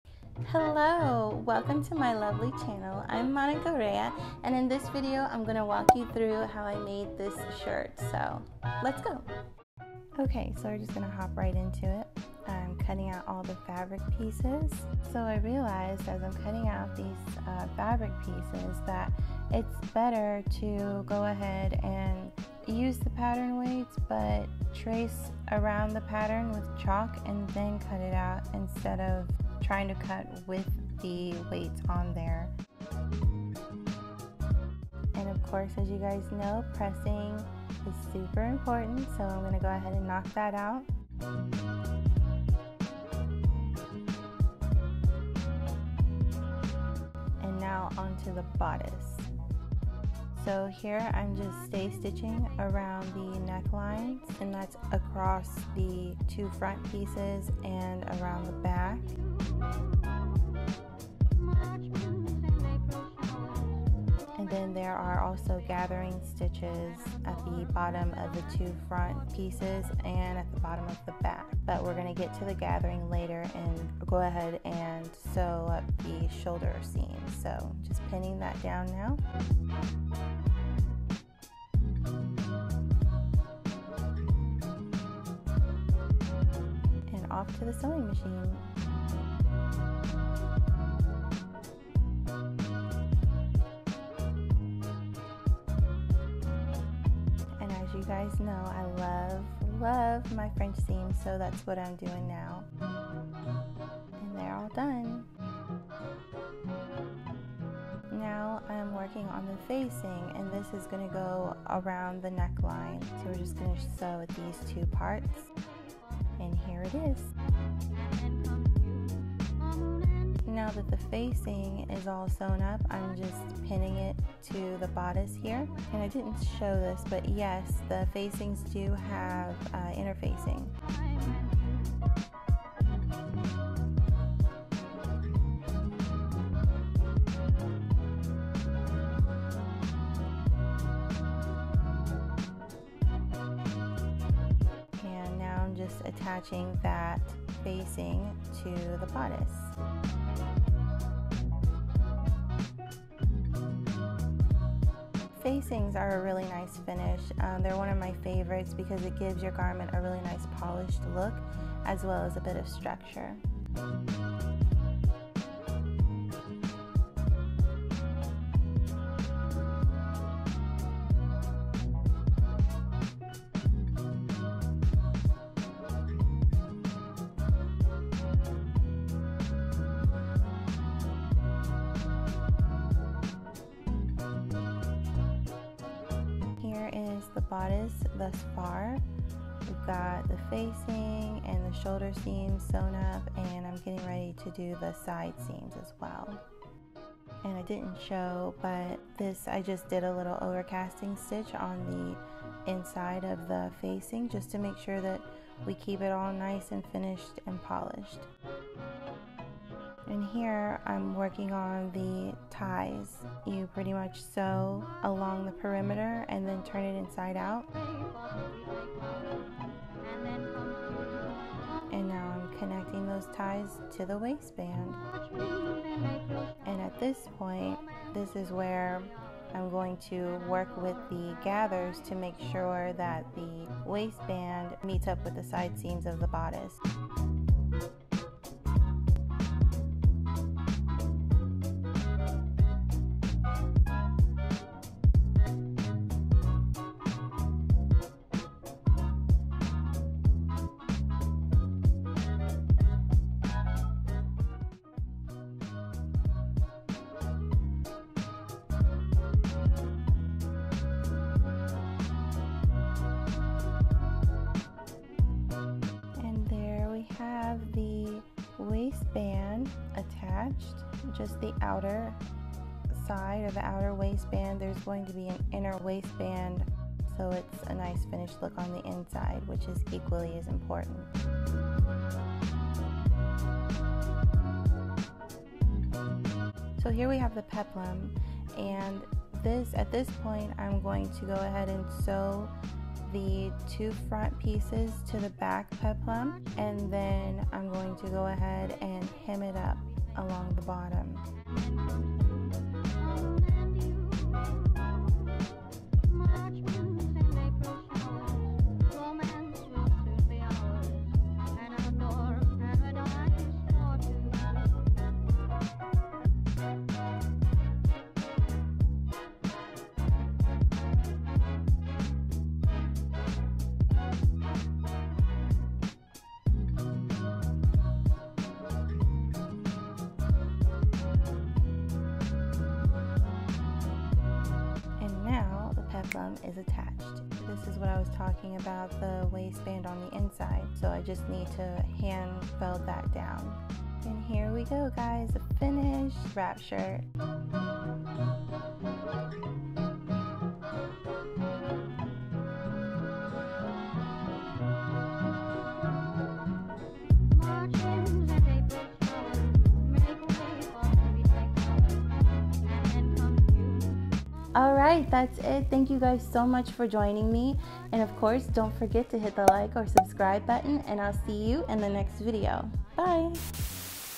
SEW WITH ME: Beautiful DIY Wrap Top | Beginner Friendly
In this video, I'll be constructing a fun and easy DIY wrap top. This style of shirt is absolutely perfect for the spring/summer months! It's also great for beginners. For this project, I'll be using the Simplicity Pattern 8644. The fabric is a beautiful lightweight linen lyocell blend. I definitely recommend trying this pattern out, the fit is quite nice. Everyone should have a good wrap top in their closet.

0:00 - Intro
0:10 - Cutting
0:33 - Pressing
0:49 - Bodice
3:46 - Waistband
4:47 - Peplum
5:43 - Unveiling